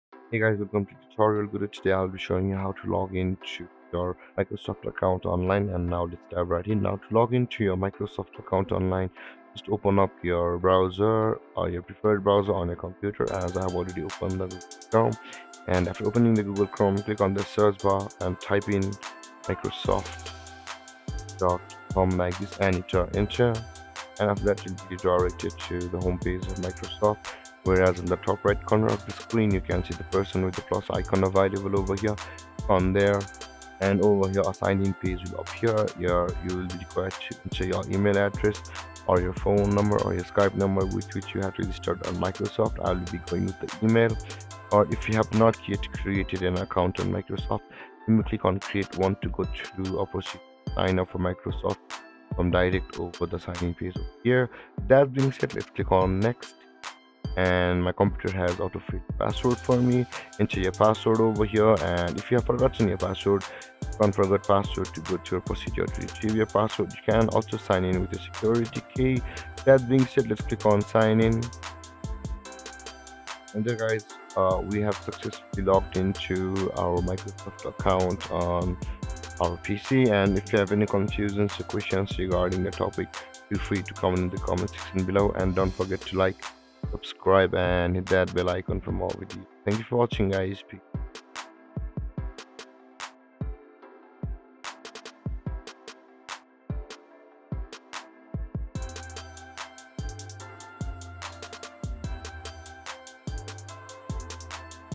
How to Login Microsoft Account? Hotmail Login | Outlook Login | Hotmail Sign In
Learn how to login to your Microsoft account on @TutorialGurudev. You can easily log in to your Microsoft account online to receive your outlook mails online & to be able to access your Hotmail data. This video will show you how to login to your Microsoft account with quick and easy steps. So, watch till the end for all the steps.